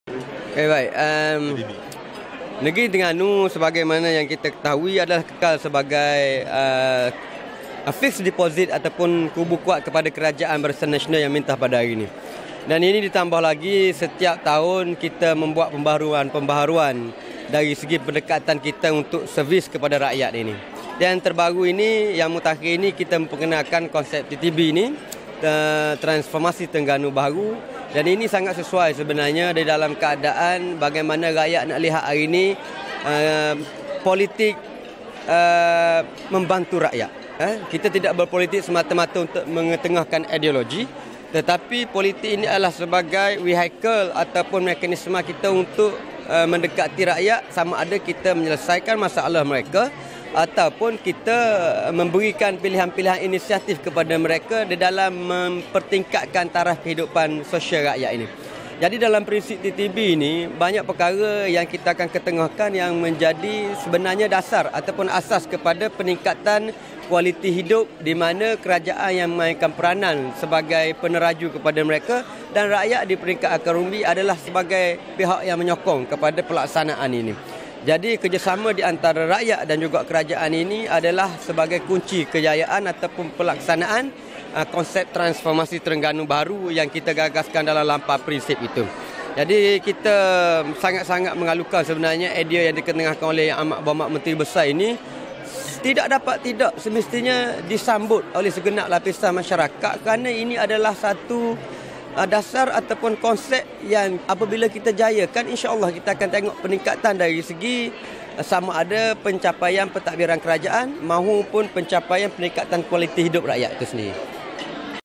YB Mohd. Iskandar Jaafar Exco Pemuda UMNO di
YB Mohd. Iskandar Jaafar  Exco Pergerakan Pemuda UMNO Malaysia merangkap Adun Jabi Setiu Terengganu di Perhimpunan Agung UMNO 2015 di PWTC Kuala Lumpur.